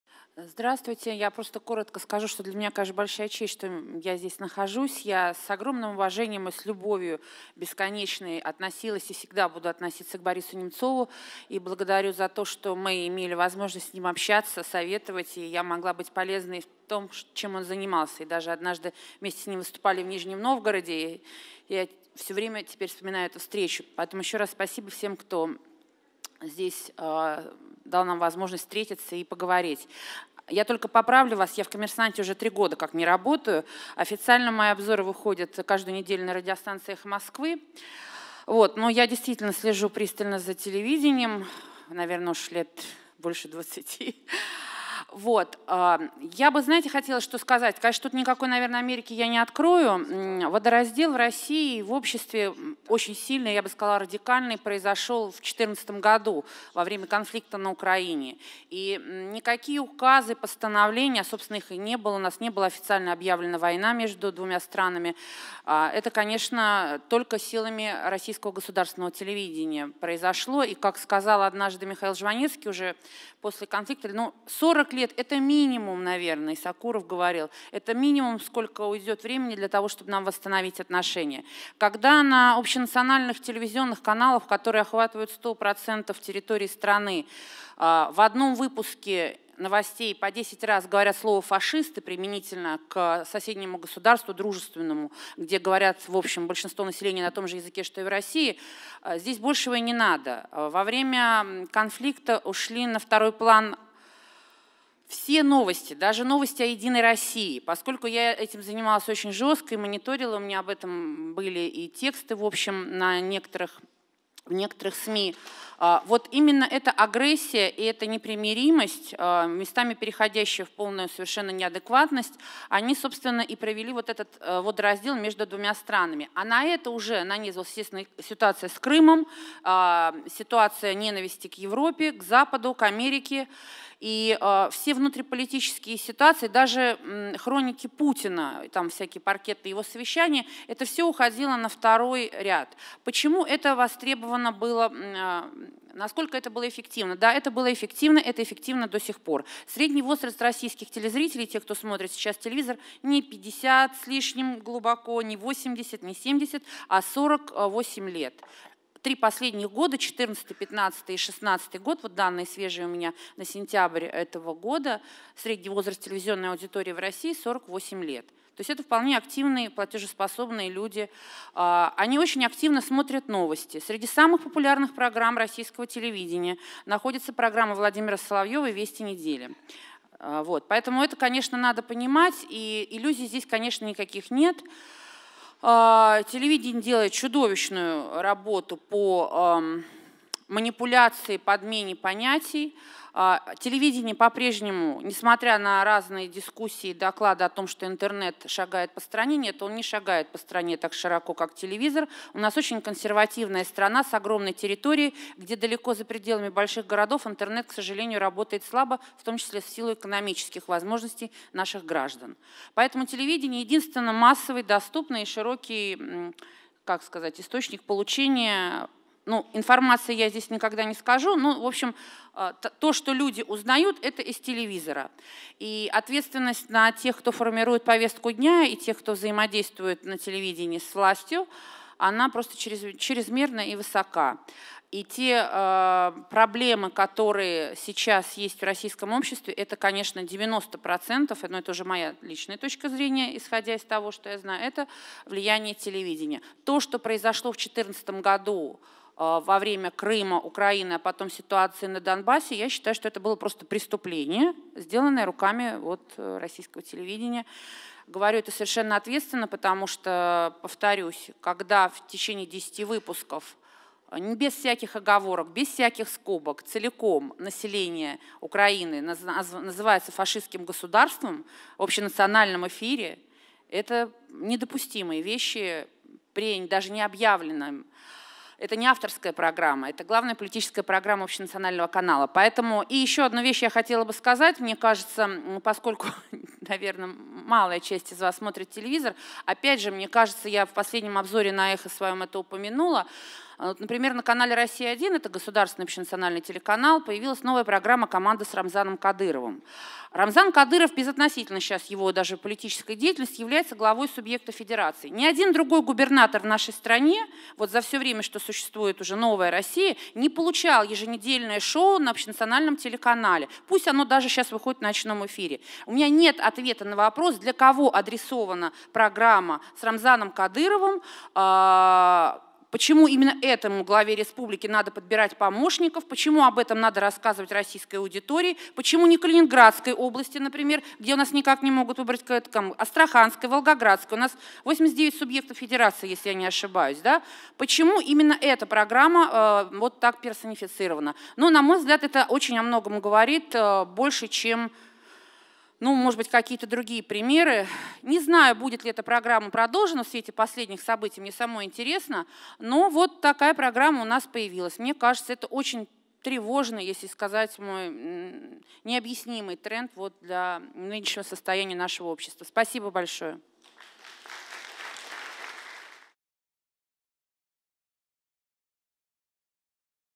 Boris Nemtsov Forum-2016. 3-я сессия. Дискуссия. Арина Бородина
3-я сессия: Разные пути вопреки общим ценностям?
Комментарий независимого телеобозревателя Арины Бородиной.